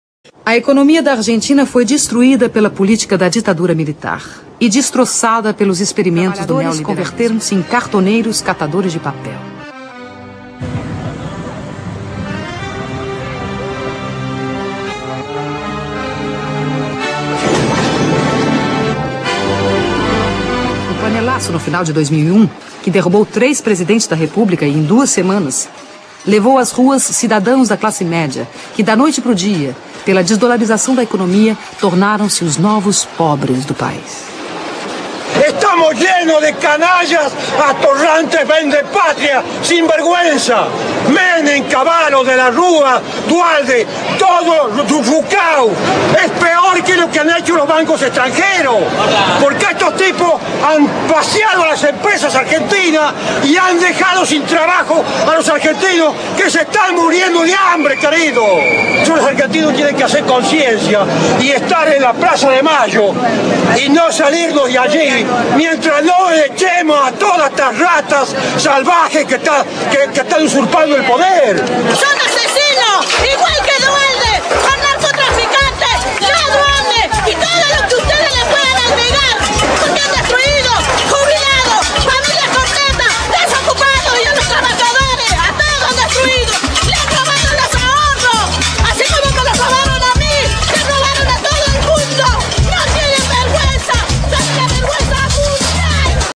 Classes médias
O caso da Argentina. Em que as classes médias apoiaram ou deixaram andar o poder, a política neoliberal apesar de as classes trabalhadores e a economia estarem a ser fortemente penalizadas.

As classes médias só compreenderam o logro quando foram arruinadas e aí, mobilizaram-se então, provocando grandes mudanças no país...